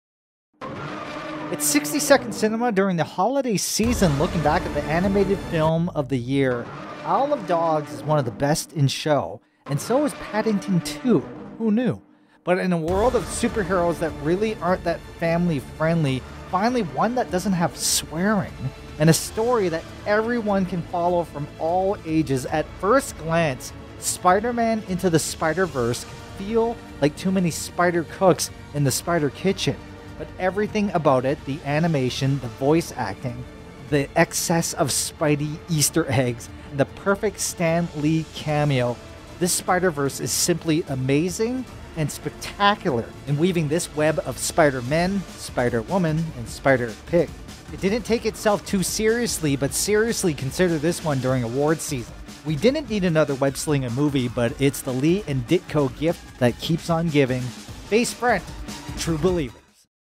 OFF AIR - "60 Second Cinema - Spider-Man Into the Spider-Verse"
The surprise of the year is courtesy of your Friendly Neighborhood Spider-Man.

A Rick Lee Production